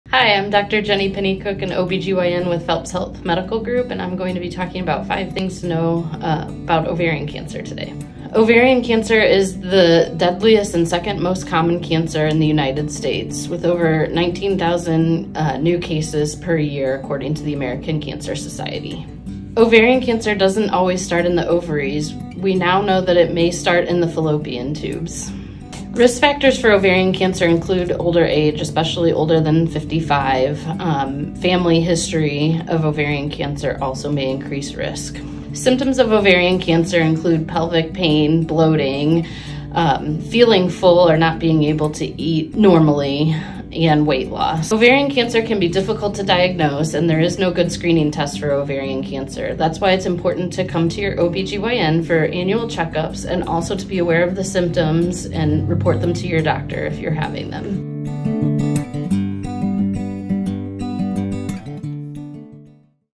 Five Things to Know About Ovarian Cancer | Phelps Health
Dr. Jenny Pennycook, an OB/GYN with Phelps Health shares five things to know about ovarian cancer.